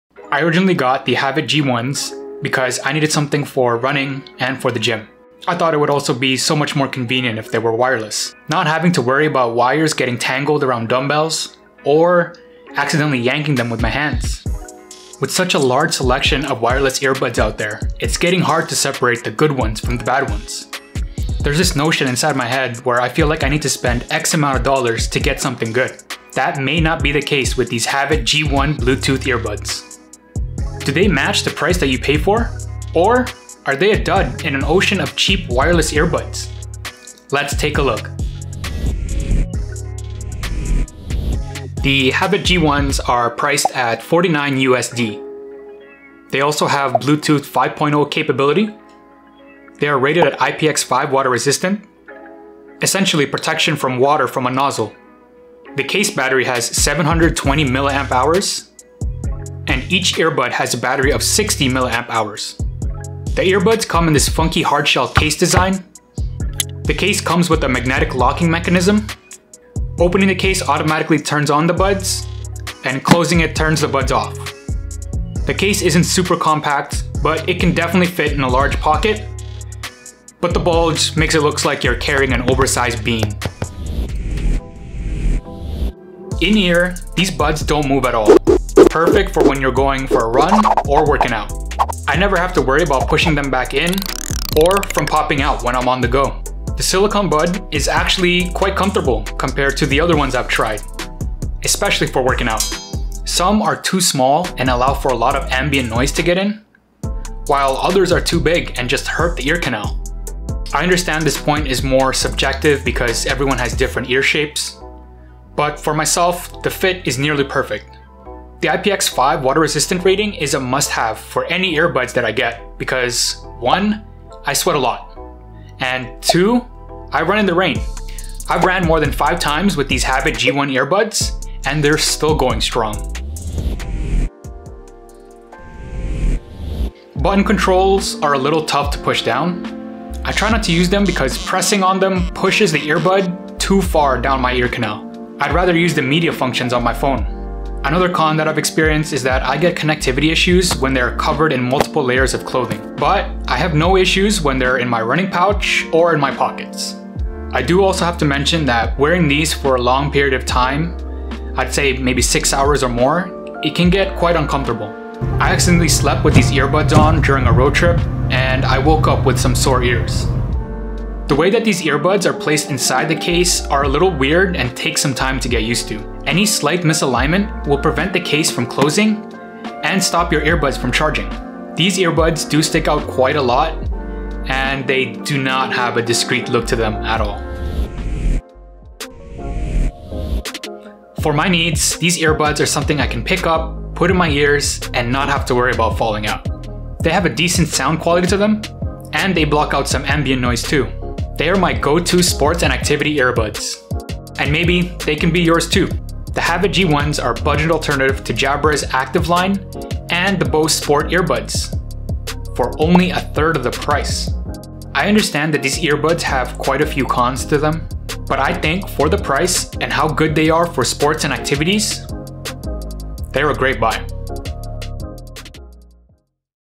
Havit Wireless Earbuds Review | My Earbuds for the GYM and RUNNING
Havit G1 Wireless Bluetooth Earbuds Review. These earbuds have been my workout earbuds for the gym, Ill let you know how they worked for me so you can decide if they are for you. I check out the functions, the pros, the cons, and my final thoughts. Are the Havit G1 wireless bluetooth earbuds the best for running and working out?